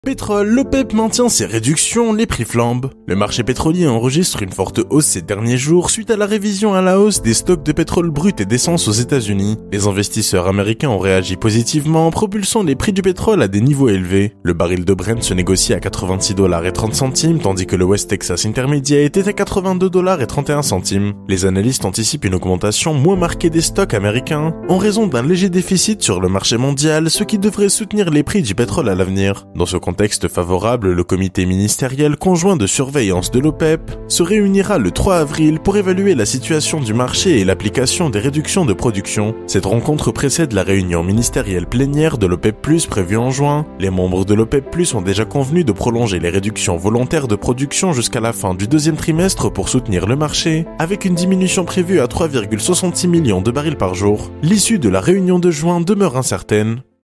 Pétrole : l’Opep maintient ses réductions, les prix flambent
Le pétrole traverse une phase de hausse importante ces derniers jours. Ce jeudi 28 mars, les cours ont encore augmenté en raison de la réévaluation par les investisseurs des dernières données sur les stocks de pétrole brut et d'essence aux États-Unis.